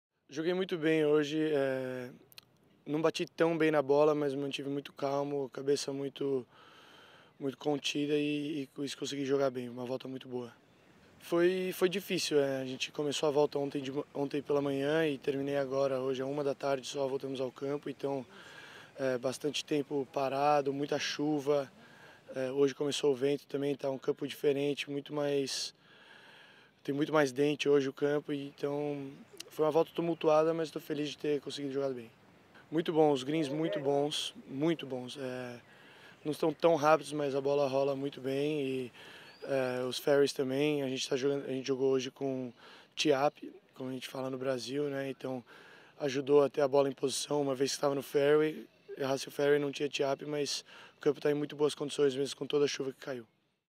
Rafa Becker fala da sua 1ª Volta no Personal Classic 2015
Depois de jogar 6 abaixo do par (66) para ficar na liderança do Personal Classic apresentado por NEC, o brasileiro Rafa Becker falou sobre seu jogo no CCG Las Praderas.